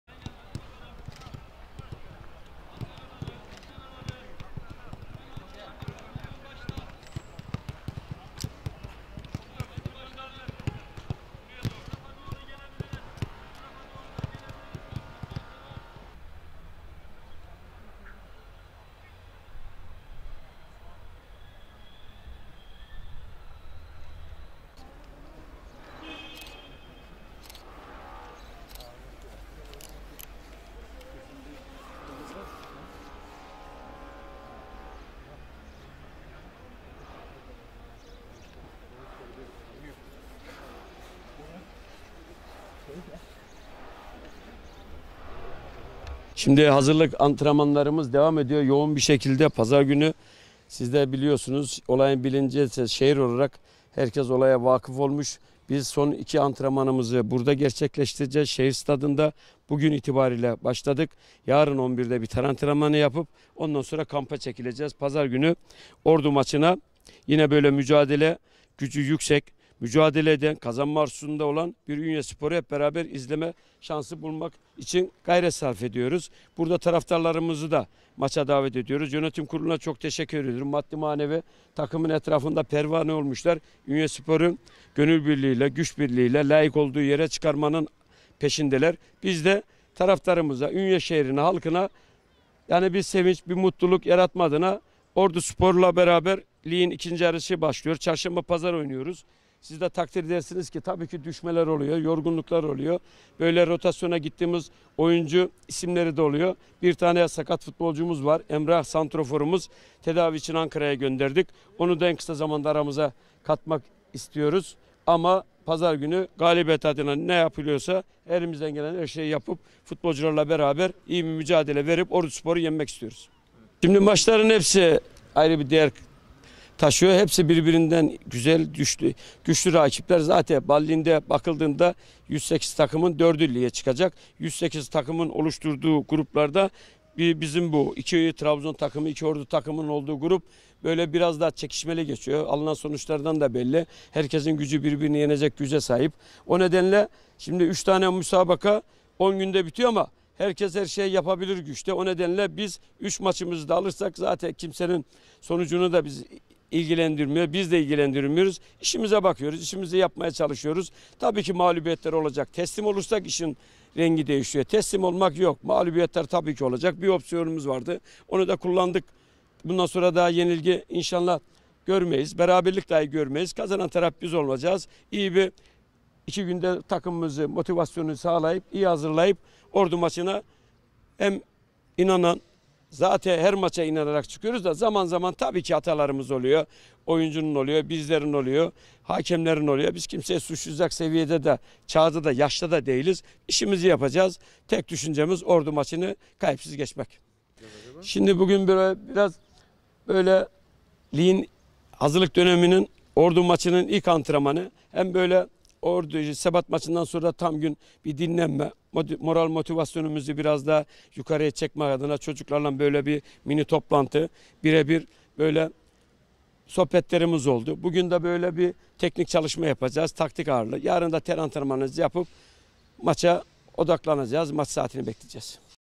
MÜCADELEMİZ ÜNYELİLERE SEVİNÇ VE MUTLULUK YAŞATMAK
Ünye 1957 Spor 19 Haziran Pazar günü kendi evinde oynayacağı Orduspor 1967 derbisine şehir stadında yaptığı teknik ve taktik antrenmanlarla hazırlanıyor.
Teknik Direktör Aydın Yavuz, Şehir Stadındaki antrenmanların 2 gün süreceğini ifade ederek, ilk gün teknik ve taktik antrenmanlar ikinci gün ise ter antrenmanının ardından kampa gireceklerini ifade etti.
Yavuz yaptığı açıklamada, “Son iki antrenmanımızı Ünye Şehir stadında gerçekleştireceğiz. Bugün itibari ile başladık. Cumartesi 11:00’da ter antrenmanı yaparak kampa çekileceğiz. Pazar günü Ordu maçına mücadele gücü yüksek, kazanma arzusunda olan bir Ünye 1957 Spor’u hep beraber izleme şansı bulmak için gayret sarf ediyoruz.” dedi.
Ünye adına mücadele ettiklerini vurgulayan Yavuz, “Yönetim kurulu maddi ve manevi takımın etrafında pervane olmuşlar. Ünye 1957 Spor’u gönül ve güç birliği layık olduğu yere çıkarmanın peşindeler. Bizler taraftarımıza, şehre, Ünye halkına sevinç ve mutluluk yaşatma adına mücadele ediyoruz.” dedi.
LİG’İN 2. YARISI BAŞLIYOR
Bu hafta oynanacak Orduspor karşılaşması ligin ikinci devresinin başlayacağını işaret eden Yavuz, “Orduspor ile ligin 2. Yarısı başlıyor. Çarşamba ve Pazar oynuyoruz. Takdir edildiği üzere düşmeler oluyor, yorgunluk oluyor. Rotasyona gittiğimiz futbolcu isimleri de oluyor. Bir tane sakat futbolcumuz var. Santraforumuz Emrah’ı tedavi için Ankara’ya gönderdik. Onu da en kısa zamanda aramıza katmak istiyoruz.” dedi.
PAROLAMIZ MUTLAK GALİBİYET
Pazar günü mutlak galibiyet parolası ile sahaya ineceklerinin altını çizen Yavuz açıklamasında şunları kaydetti, “Pazar günü galibiyet adına ne yapmak gerekiyorsa elimizden gelen her şeyi yapıp Orduspor’u yenmek istiyoruz.   Karşılaşmaların her biri ayrı bir değer taşıyor. Hepsi bir birinden güzel güçlü rakipler. BAL ligine bakıldığında 108 takımın 4’ü lige çıkacak. Bizim içimizde bulunduğumuz 2 Trabzon, 2 Ordu takımının bulunduğu lig biraz daha çekişmeli geçiyor. Herkes bir birini yenecek güce sahip. Biz 3 maçımızı alırsak hiç kimsenin sonucu bizi ilgilendirmiyor. Biz işimizi yapıyoruz. Tabi ki mağlubiyetler olacak. Ama teslim olursak işin rengi değişiyor. Teslim olmak yok. Bir opsiyonumuz vardı. Onu da kullandık. Bundan sonra inşallah yenilgi ve hatta beraberlik görmeyiz.”